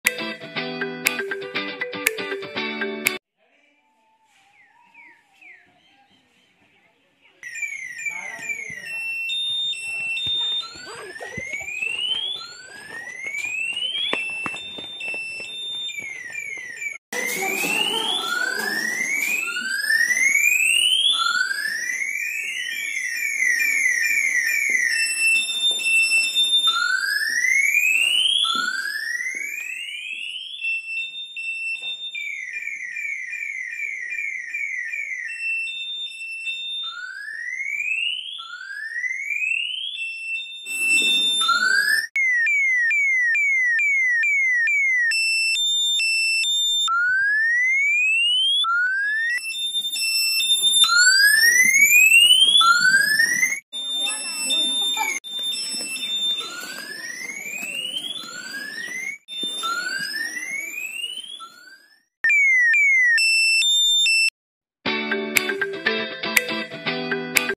Mock drill for Earthquake
How can we protect our life during natural disaster such as Earthquake.